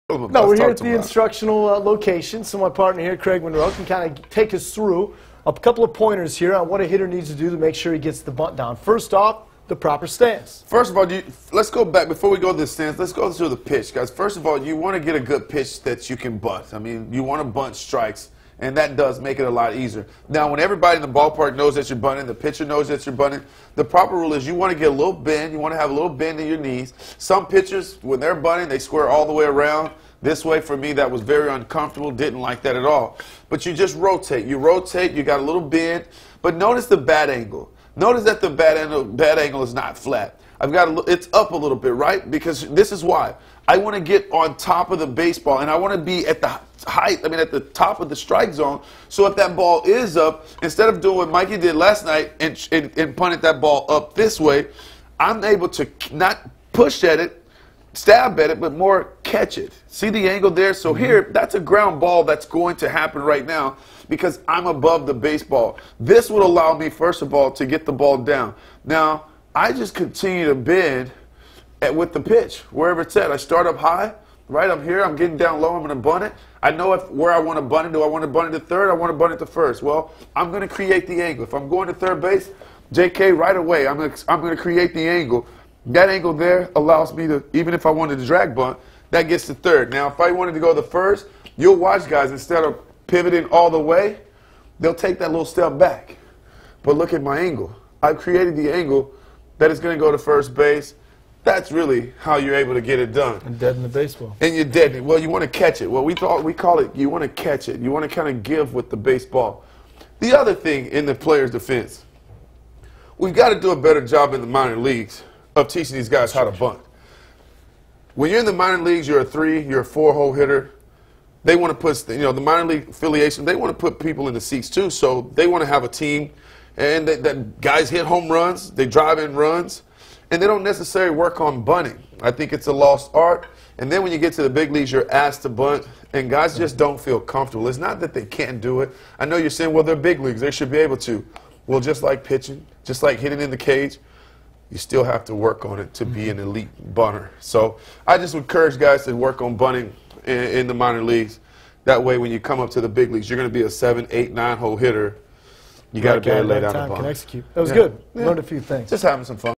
The lost art of bunting
Craig Monroe demonstrates the right way to lay down a bunt.